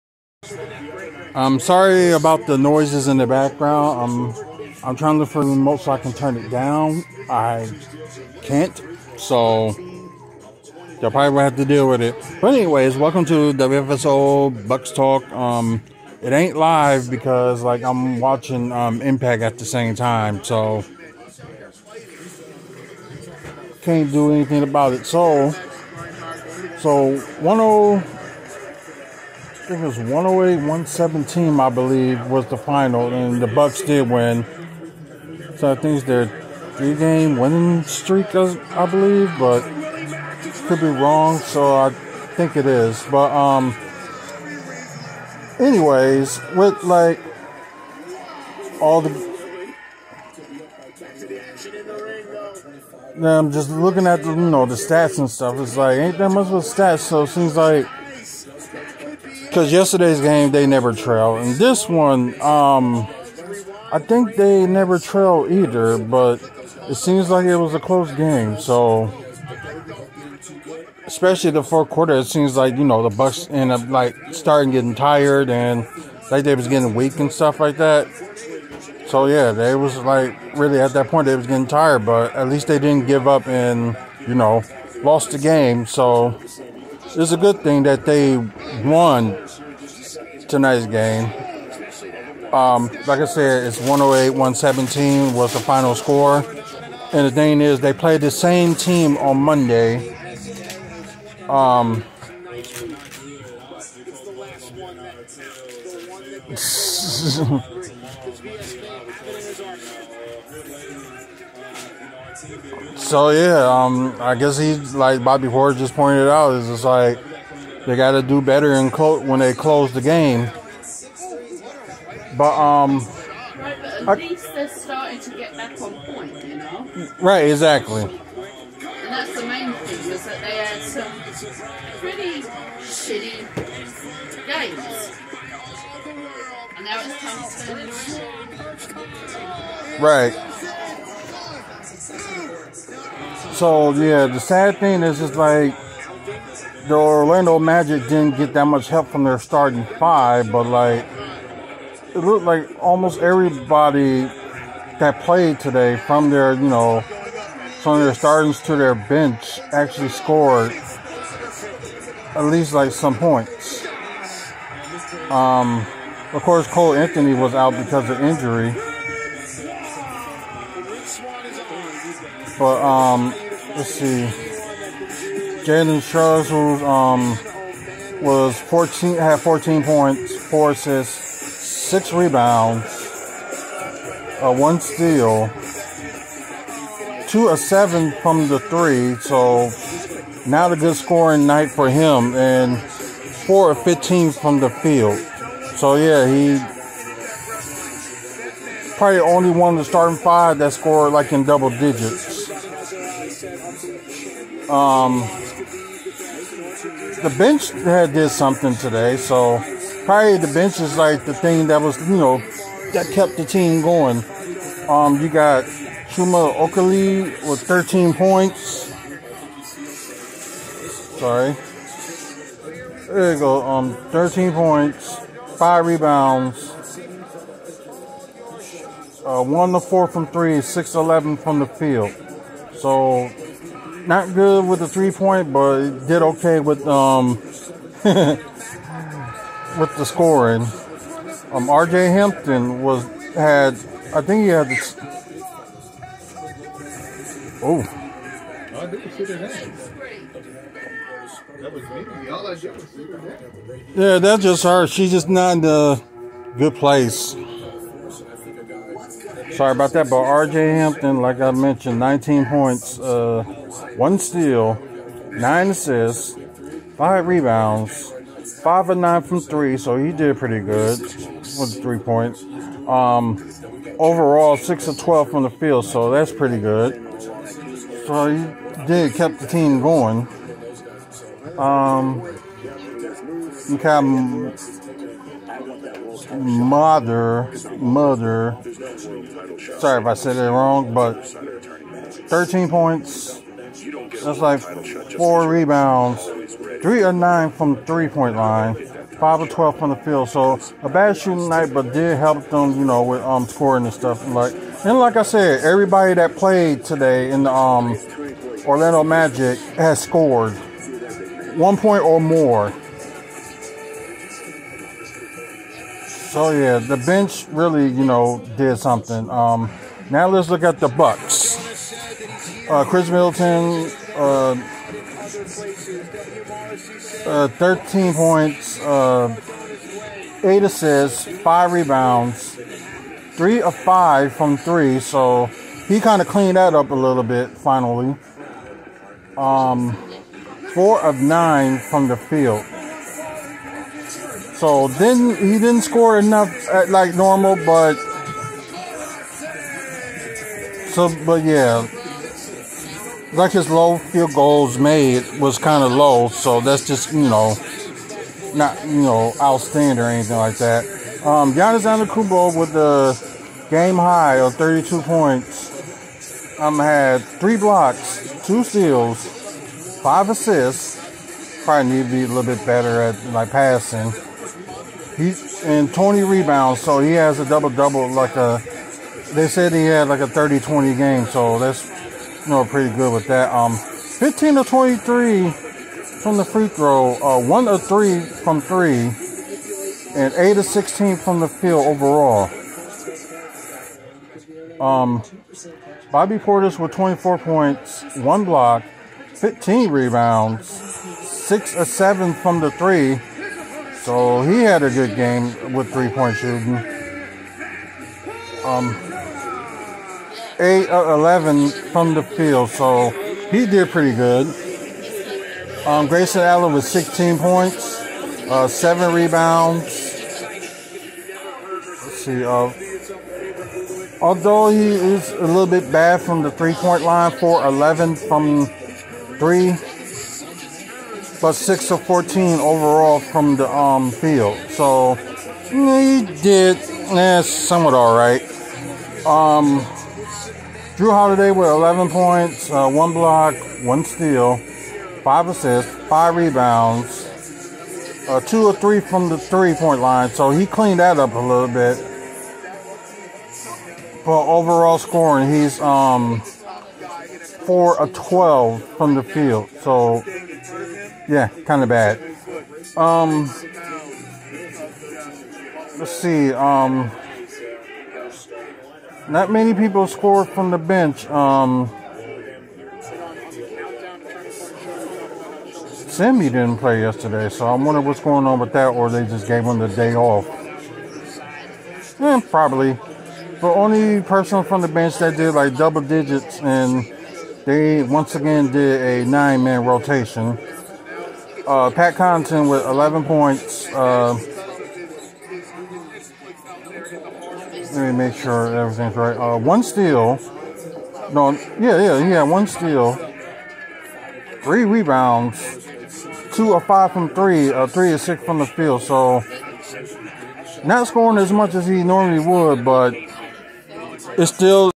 WFSO Bucks Talk: Magic vs Bucks 11/20/21
Artrell talks about Magic vs Bucks.